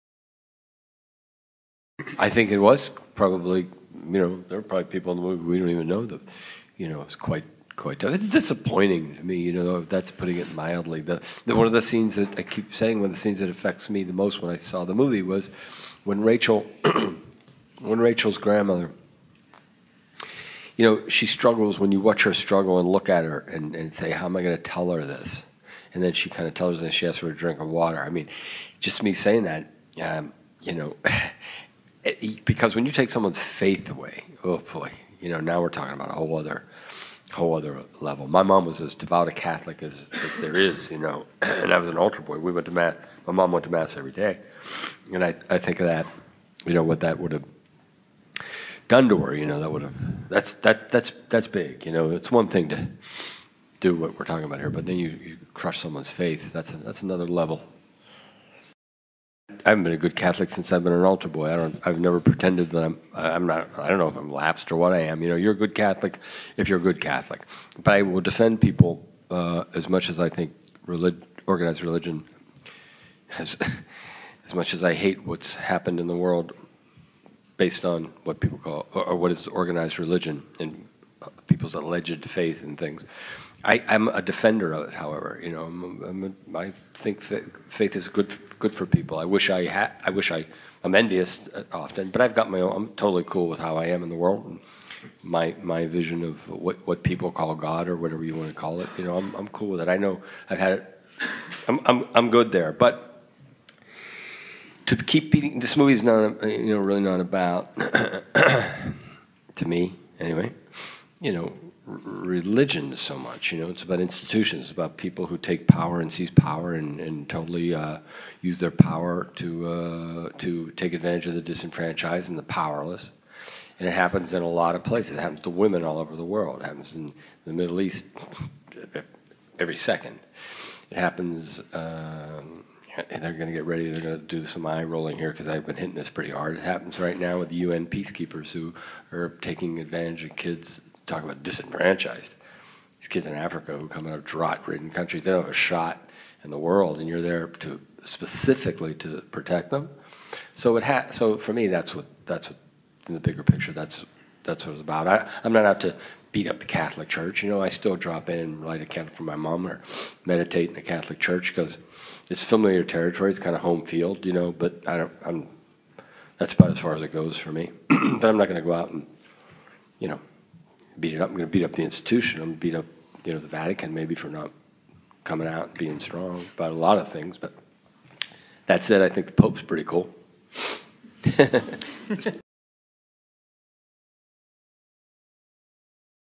Michael Keaton Interview - Catholic Faith & Sex Abuse Scandals
Mark Ruffalo, Michael Keaton, Director Tom McCarthy and real life journalists Mike Rezendes  & Walter Robsinon talk  at the London press conference for Oscar nominated drama Spotlight. The true story of how the Boston Globe uncovered the massive scandal of child molestation and cover-up within the local Catholic Archdiocese, shaking the entire Catholic Church to its core.